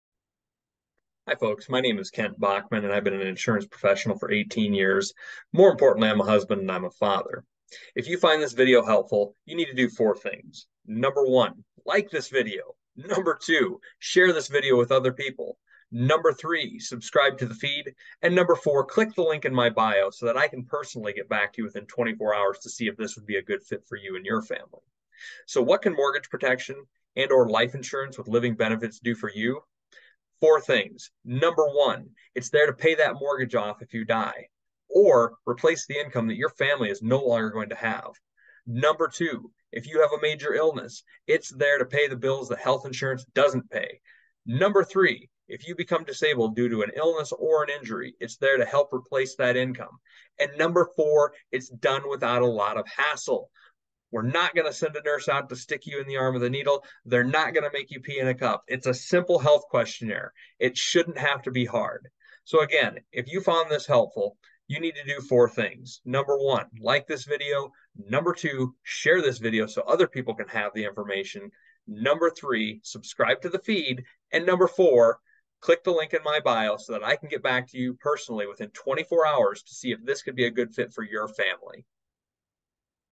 What does Mortgage Protection or Life insurance with living benefits do?
"Secure Your Family's Future! 🏠💼 Discover the Power of Mortgage Protection & Life Insurance with Living Benefits 🛡️💰 Don't miss this eye-opening YouTube video that reveals how these essential financial tools can safeguard your home, provide financial stability, and protect your loved ones in times of need. Act now and make the smart choice for your family's well-being!